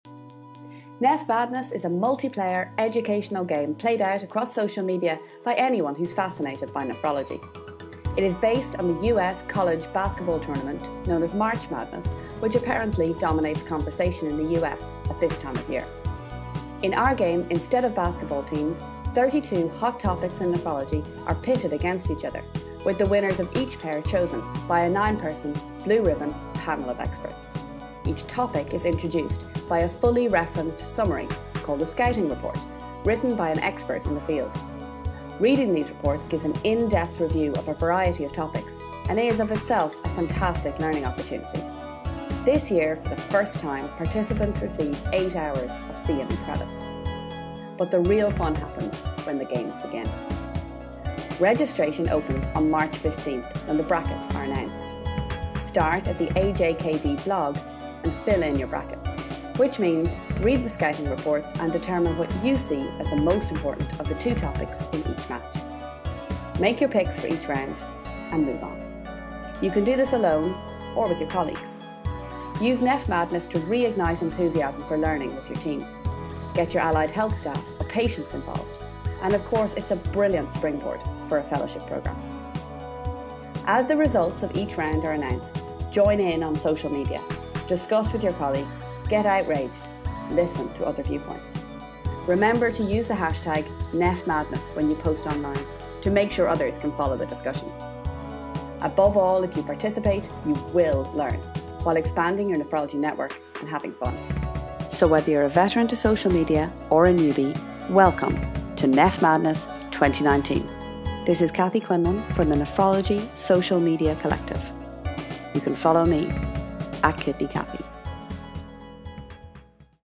What is this madness 2019
Nephmadness is an educational game for anyone interested in nephrology, opening on March 15th 2019
It's a hugely enjoyable way to learn loads while connecting with nephrologists across the globe.This video explains how to play.